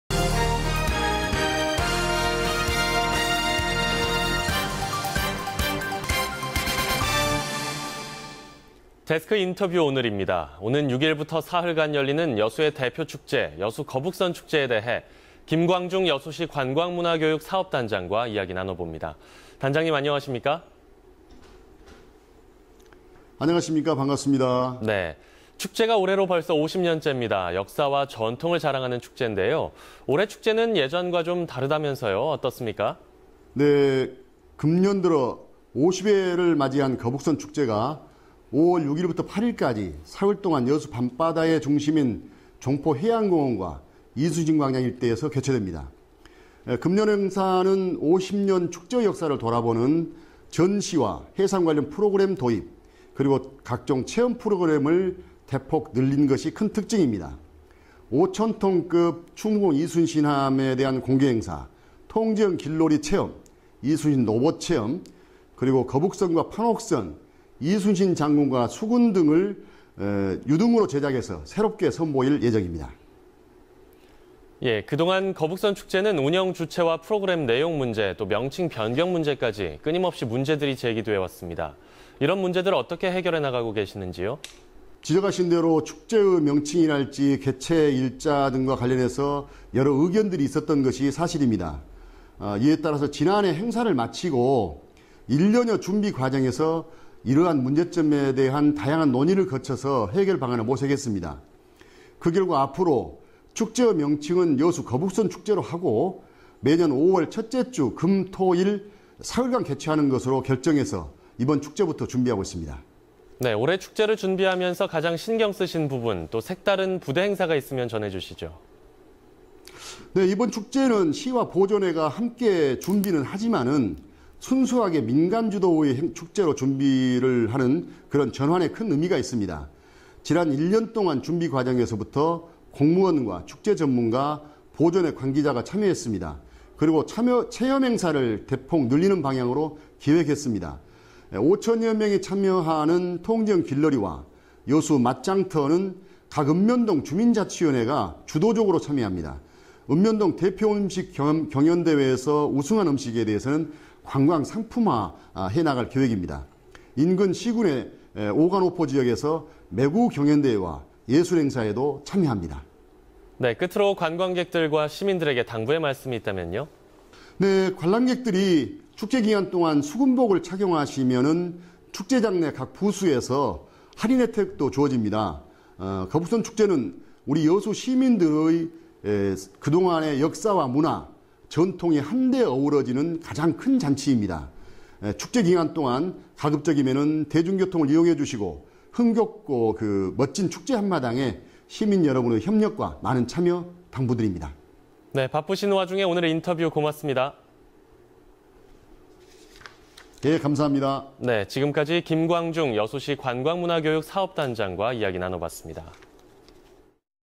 [데스크인터뷰]50주년 맞은 거북선축제 - 김광중 여수시 관광문화교육사업단장 (160502월)
데스크 인터뷰 오늘 입니다. 오는 6일부터 사흘간 열리는 여수의 대표 축제, 여수 거북선 축제에 대해 김광중 여수시 관광문화교육사업단장님과 함께 이야기 나눠 보겠습니다.